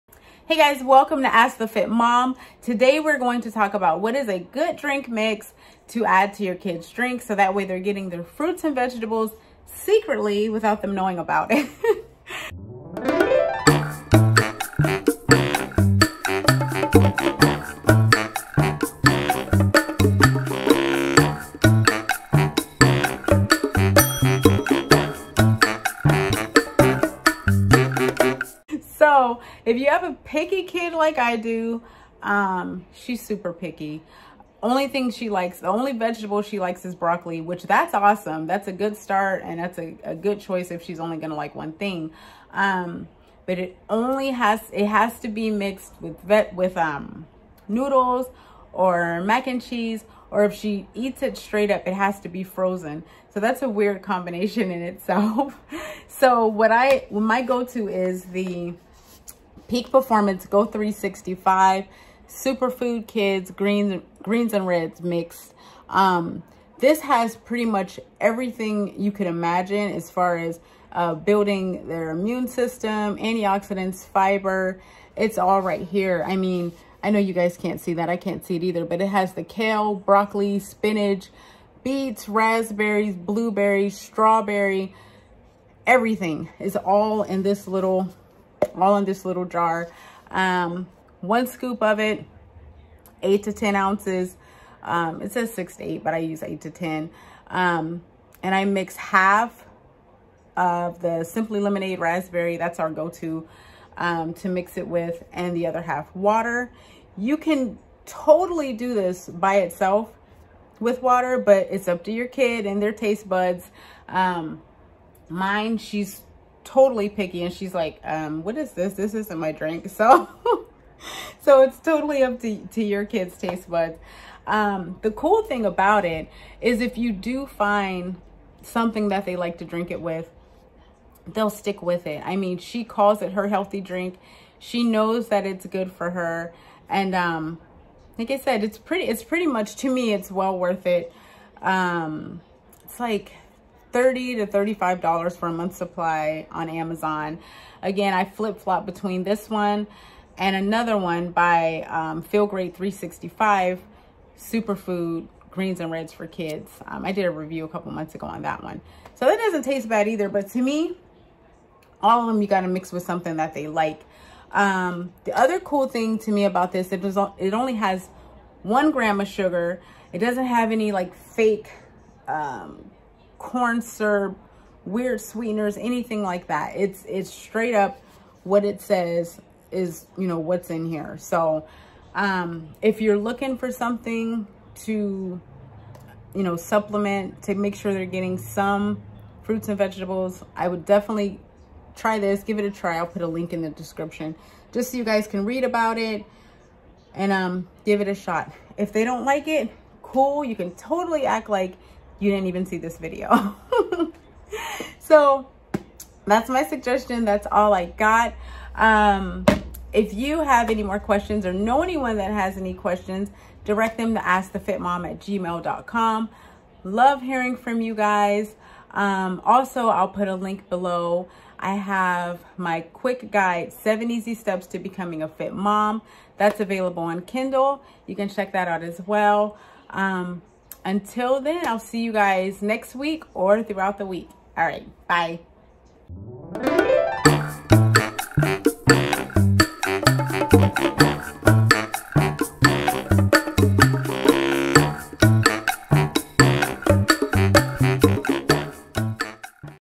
SuperFood Drink For Kids? – Easy Way To Get Your Kids Their Fruits And Veggies! Ask The Fit Mom Ep 7
★★ Delicious Red & Green SuperFood For Kids

💪★Checkout "7 Easy Steps To Becoming A Fit Mom!"

In this weeks Ask The Fit Mom I am going to be answering the question in regards to healthy drink powders for kids. Their are several options out there when it comes to super food drink mixes for kids and in this video I am going to share with you the one that I give my daughter every day!

💪★Ask The Fit Mom - Send Questions: AskTheFitMom(at)gmail.com